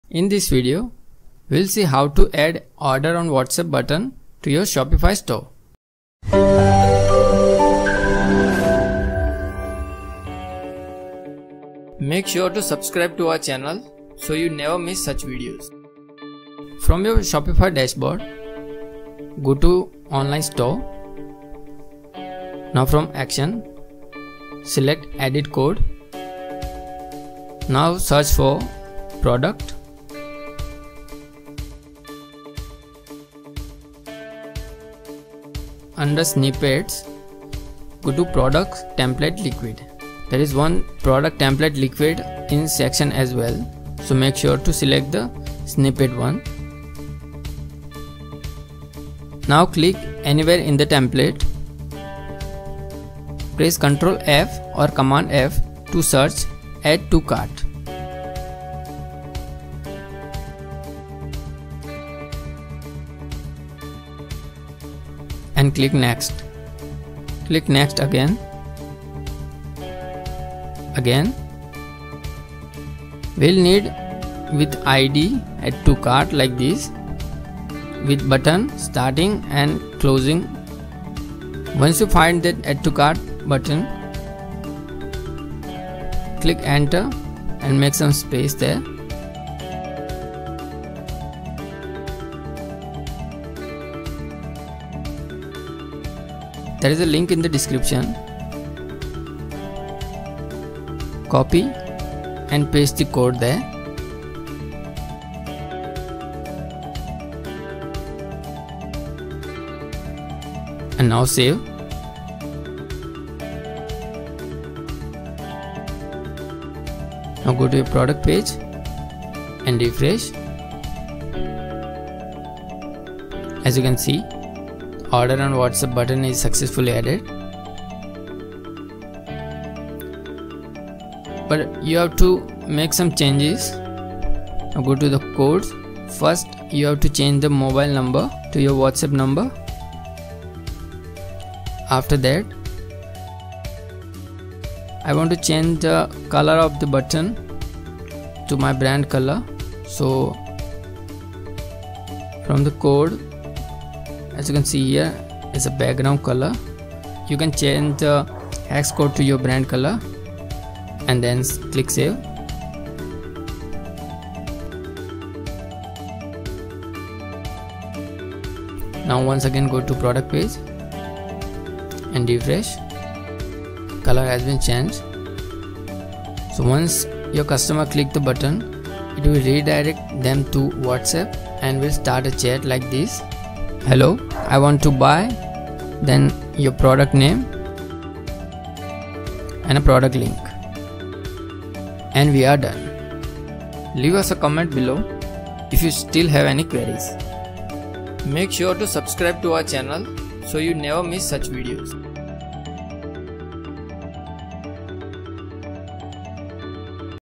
Add Order on Whatsapp Button | Shopify
❤️ Hello Everyone
In this video, we'll see how to add the "Order on Whatsapp" Button to your Shopify store.

Don't Have a Shopify Store?

- then from Themes - Go to Edit Code
👉 Step 2  ➜ Search for product-template.liquid under Snippets
👉 Step 3  ➜ Find codes of Add to Cart Button and Add the above code under that.
- Change the number to your Whatsapp Number
And Save.